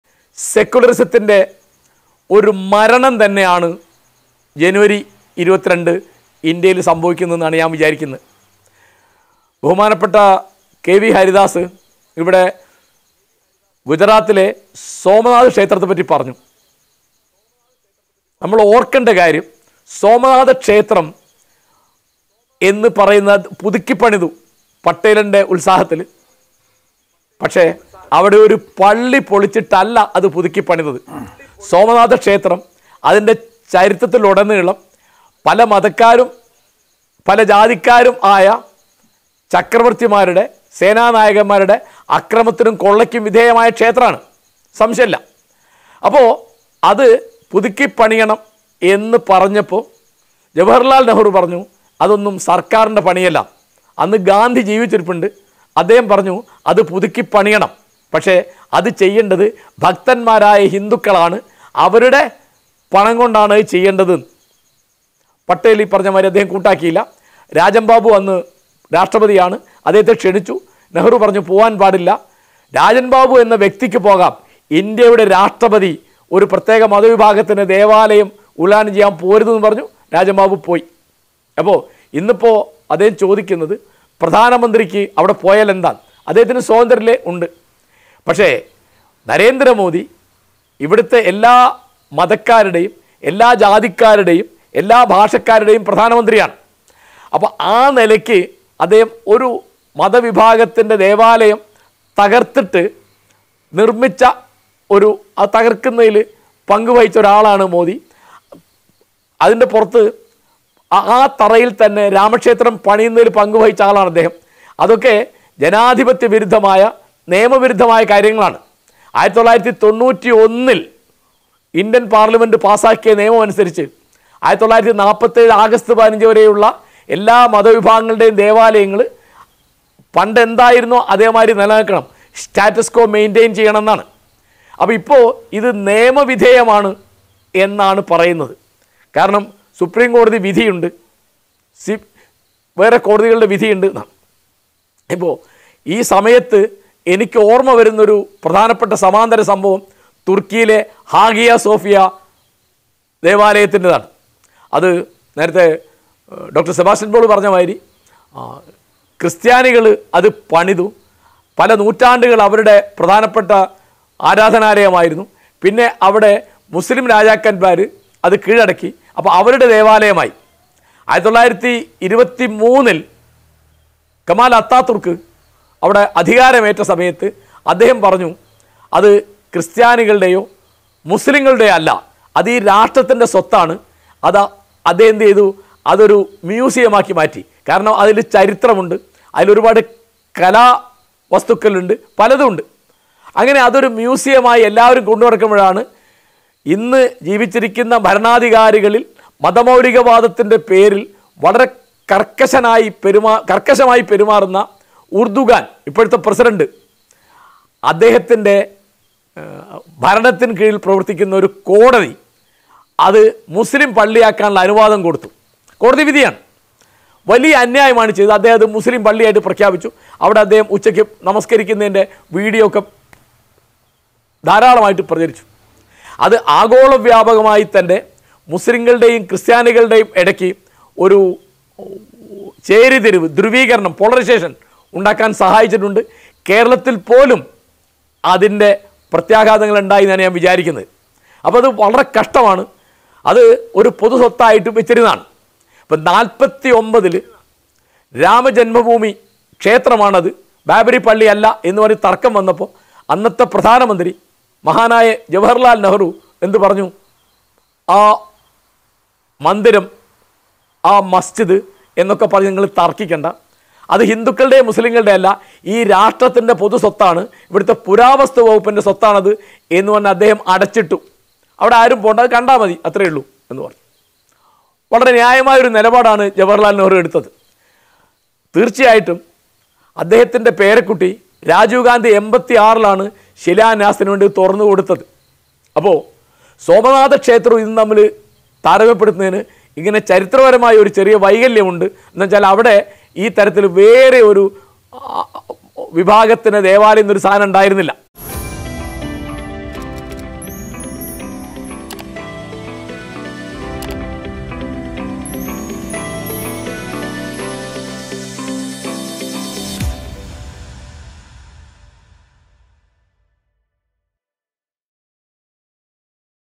'മതേതരത്വത്തിന്റെ മരണമാണ് ജനുവരി 22ന് ഇന്ത്യയിൽ നടക്കുന്നത്' | Ayodhya | Ram Mandir | PM Modi
'മതേതരത്വത്തിന്റെ മരണമാണ് ജനുവരി 22ന് ഇന്ത്യയിൽ നടക്കുന്നത്'; എല്ലാ വിഭാഗക്കാരുടെയും പ്രധാനമന്ത്രിയാണ് നരേന്ദ്രമോദിയെന്ന് എം.എൻ കാരശ്ശേരി